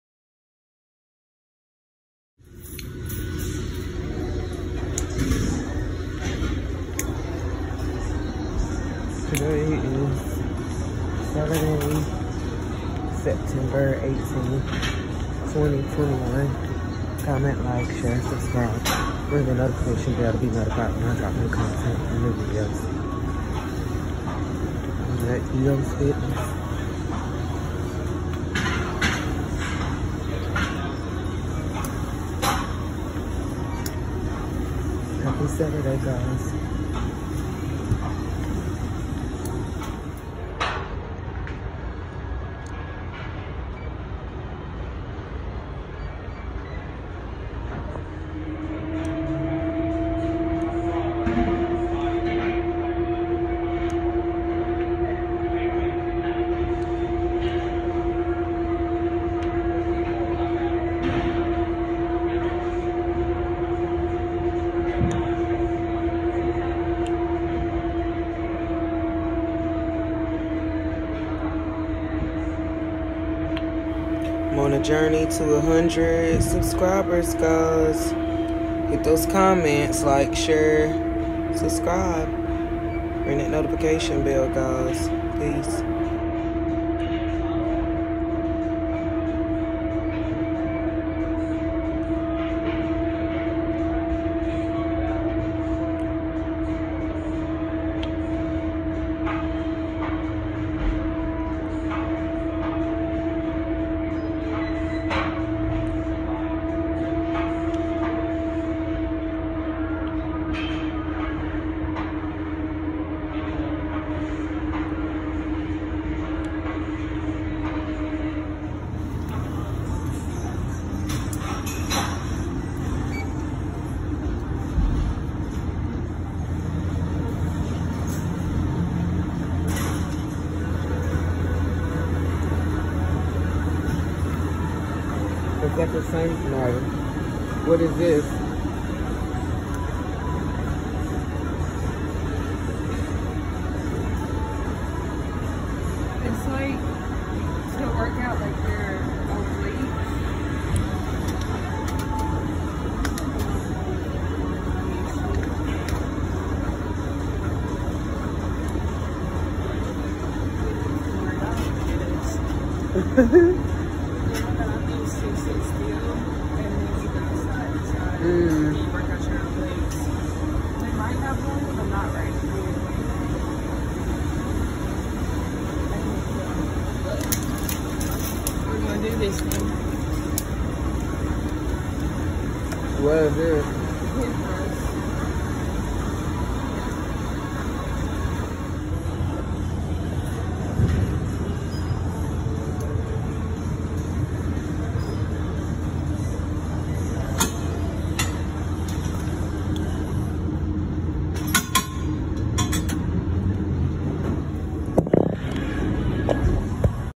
Gym Day 9-18-21 Happy Saturday!
It’s ya girl Dani!!! We went to the gym to workout! Peace and light!